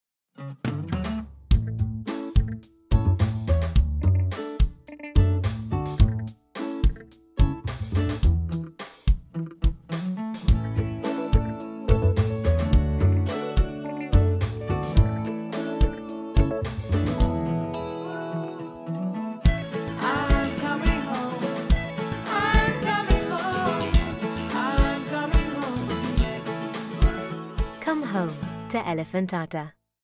Come home to Elephant Atta 30" Son Edit
Elephant Atta is launching a new brand film, following a refresh of the brand and packaging. As UK’s leading brand of atta, with over 50 years of tradition, Elephant Atta has evolved over the years, keeping up with changing times while still fulfilling the inherant need of its customers to stay connected to their roots.. Our new film taps into the modern South Asian consumer who has grown up with traditional family values, yet is very much a part of modern British life.

The film has been produced by Media Reach Advertising Ltd and Nodachi Films, and is being launched across all major South Asian TV channels. It is supported by an Outdoor, press and digital campaign.